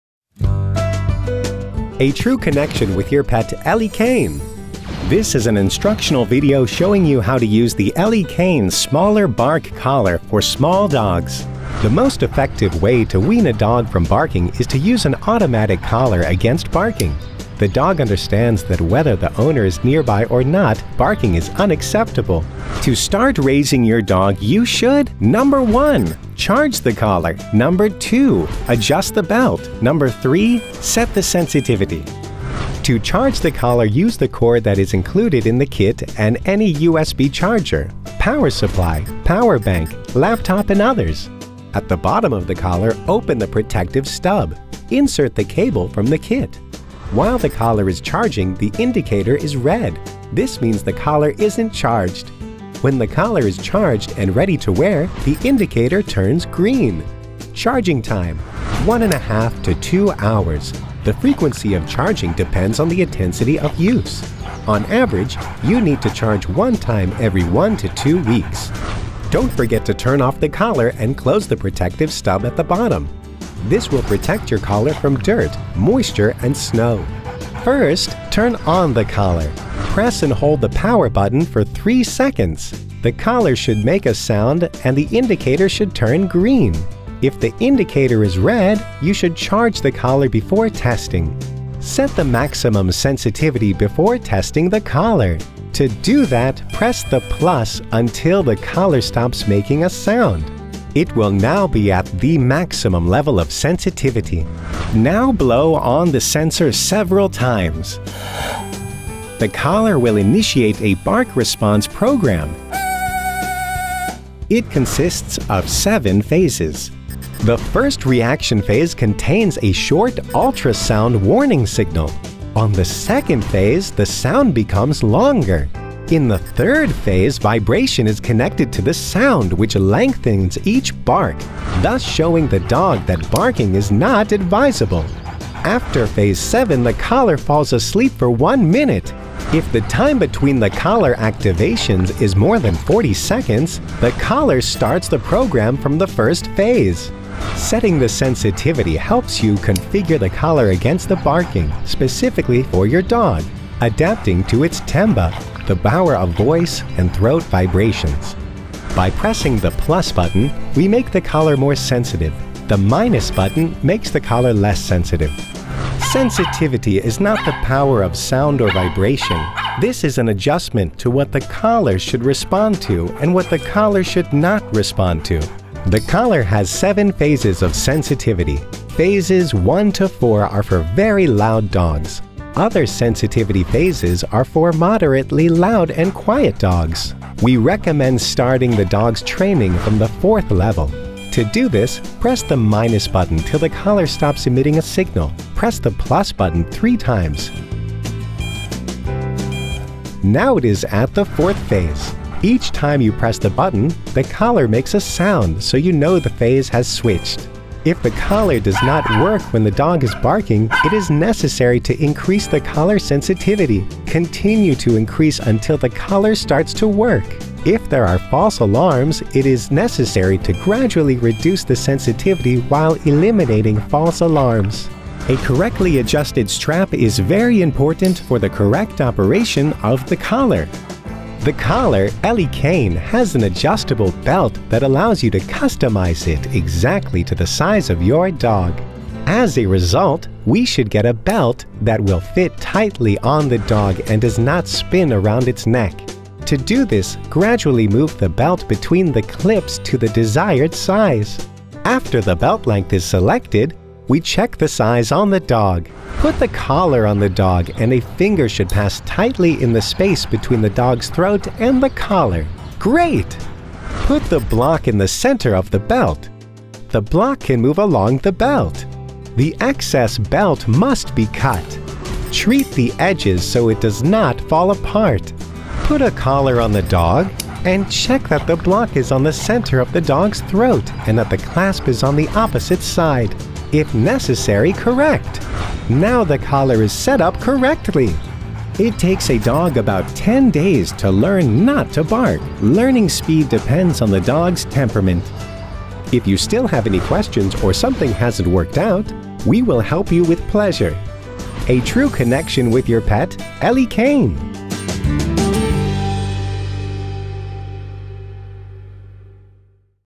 TUTORIAL SMALLEST BARK COLLAR ELECANE
This instructional video shows you how to use the Elecane smaller bark collar for small dogs.

CONTENTS:
0:32 How to charge the collar
0:58 Charging time and time to charge
1:17 How to turn on the collar
1:31 How to test the collar
2:38 What is collar sensitivity and how to adjust it
3:45 How to adjust the collar strap
4:36 How to put on a dog collar
4:49 Contact Information